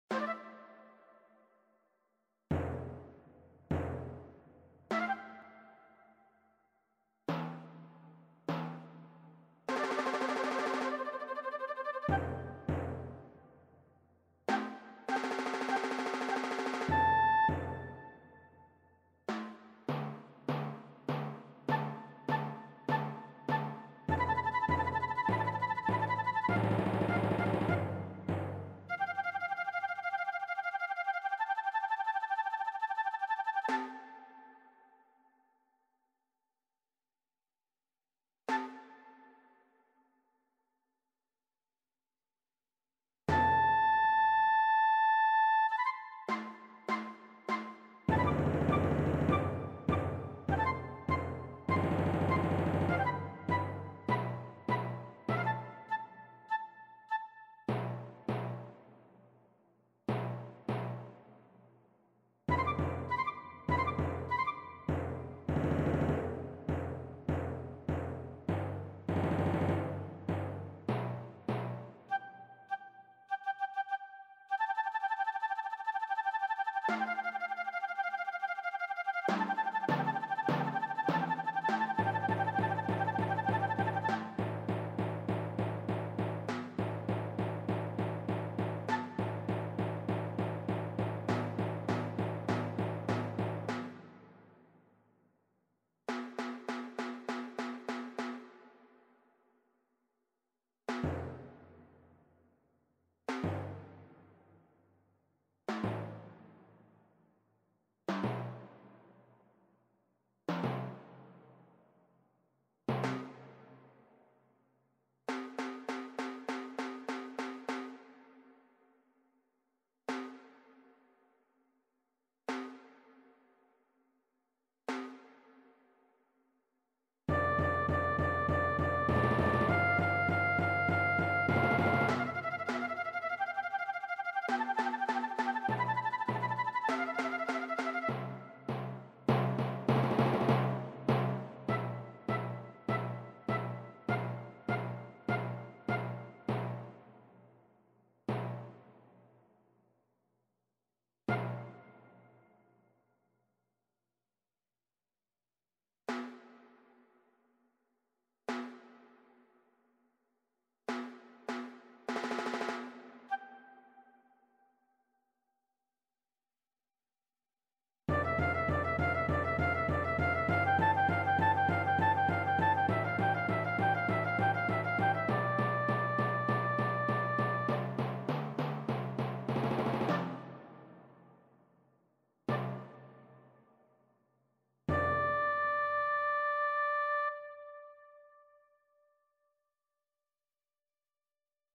My song 16
This is simple piece I made out of flute and timpani. I just said this is the "simple piece", so don't expect a lot from this piece. This was inspired by jungle and all of sounds you can hear such as mosquitoes, birds, water, and tribe.